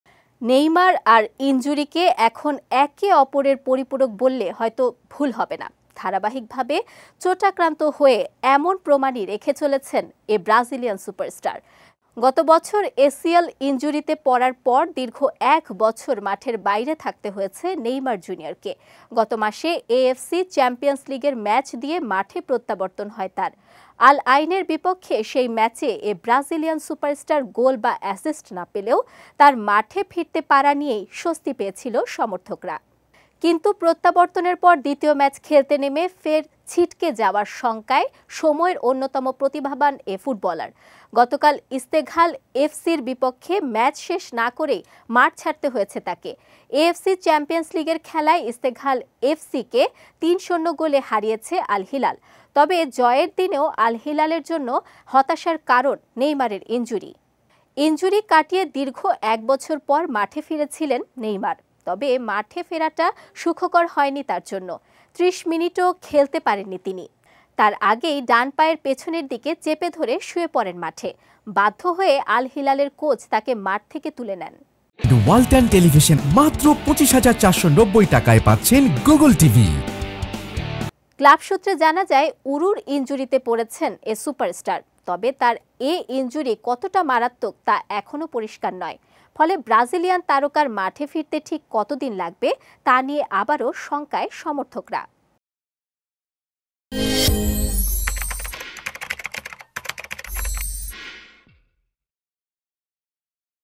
এক বছর পর মাঠে ফেরা নেইমারের নতুন ইনজুরি! Neymar | Bijoy TV
নেইমার আর ইনজুরিকে এখন একে অপরের পরিপূরক বললে হয়তো ভুল হবে না। ধারাবাহিকভাবে চোটাক্রান্ত হয়ে এমন প্রমাণই রেখে চলেছেন এই ব্রাজিলিয়ান সুপারস্টার। বিষয়টি এখন এমন যে, ইনজুরি যেন নেইমারের আত্মীয়, তাই নেইমারের পিছু ছাড়ছে না।
গত বছর এসিএল ইনজুরিতে পড়ার পর দীর্ঘ এক বছর মাঠের বাইরে থাকতে হয়েছে নেইমার জুনিয়রকে। গত মাসে এএফসি চ্যাম্পিয়ন্স লিগের ম্যাচ দিয়ে মাঠে প্রত্যাবর্তন হয় তার। আল আইনের বিপক্ষে সেই ম্যাচে এই ব্রাজিলিয়ান সুপারস্টার গোল বা অ্যাসিস্ট না পেলেও তার মাঠে ফিরতে পারা নিয়েই স্বস্তি পেয়েছিল সমর্থকরা।